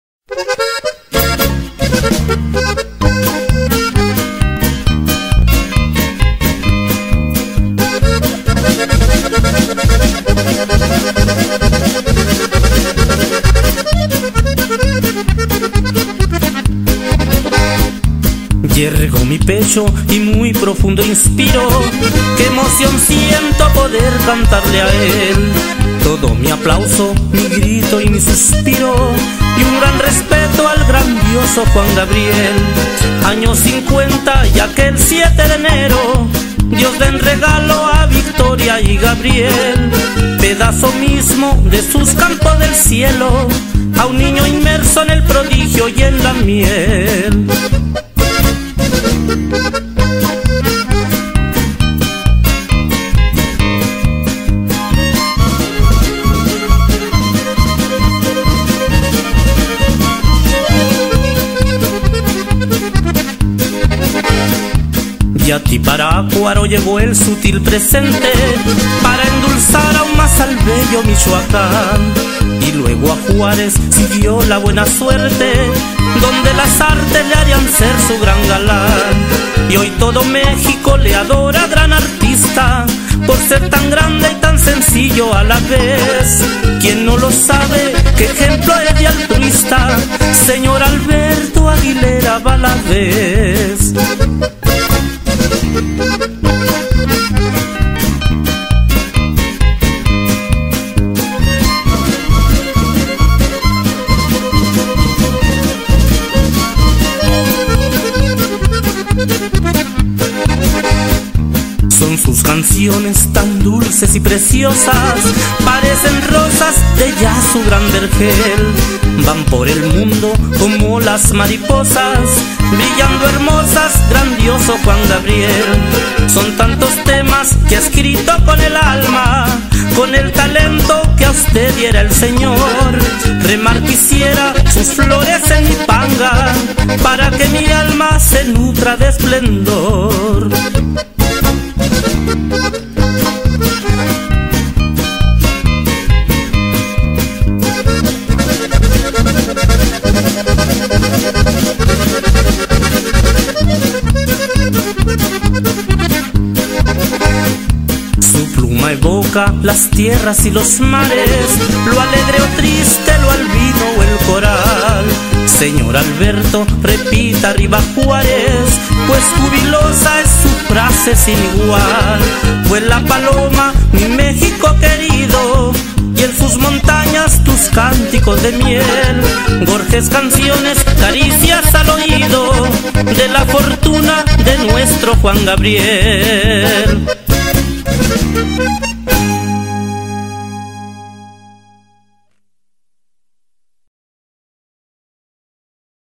JUAN GABRIEL HOMENAJE DE DANY VEGA DE HERMOSILLO
CON TODA HUMILDAD E INMENSO CARIÑO, HE REALIZADO ESTA MELODÍA EN HONOR A NUESTRO GRANDIOSO JUAN GABRIEL, ESPERO OS GUSTE !!!